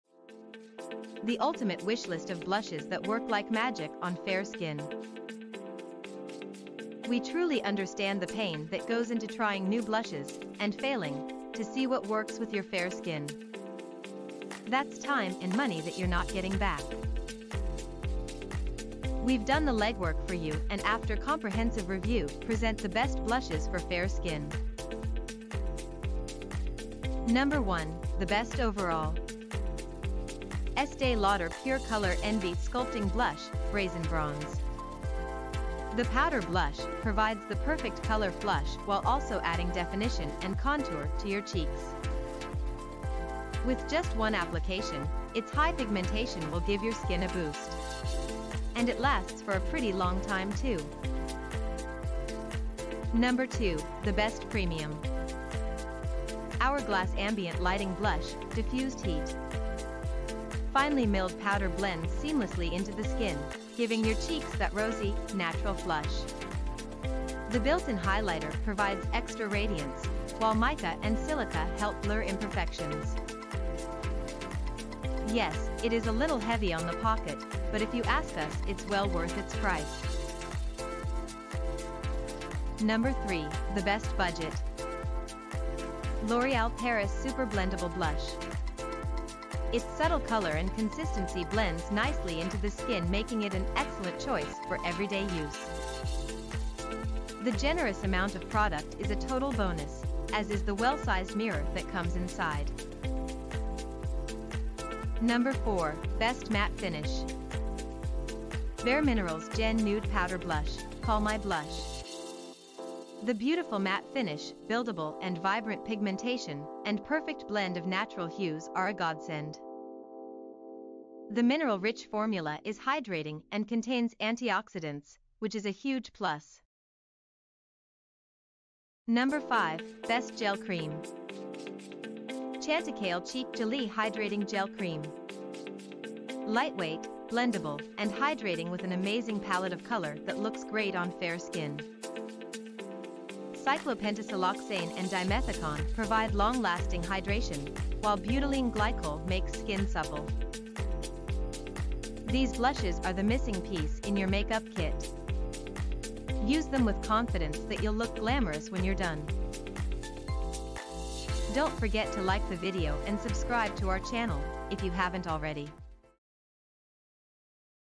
Ultimate Wishlist of Blushes for Fair Skin
When you have fair skin, finding the right blush can be a challenge. In this video, we've rounded up the best blushes for fair skin, including different options for different considerations.

Our top pick for the best overall blush is the Estee Lauder Pure Color Envy Sculpting Blush in Brazen Bronze. This blush offers buildable color and a long-lasting, natural finish that looks great on fair skin.

If you're looking for a matte finish, our top pick is the BareMinerals Gen Nude Powder Blush in Call My Blush. This blush offers a soft, natural-looking finish that's perfect for everyday wear.

For a gel-cream formula that hydrates while it colors, we recommend the Chantecaille Cheek Gelée Hydrating Gel Cream. This blush adds a natural-looking flush to fair skin while providing hydration and nourishment.

If you're willing to splurge, the Hourglass Ambient Lighting Blush in Diffused Heat is a great option. This blush features a unique lighting effect that enhances the natural radiance of fair skin.

And for those on a budget, the L'Oreal Paris Super-Blendable Blush is a great choice. This blush blends easily and offers buildable color that looks natural on fair skin.

For more information and detailed review, check out our blog post on the best blushes for fair skin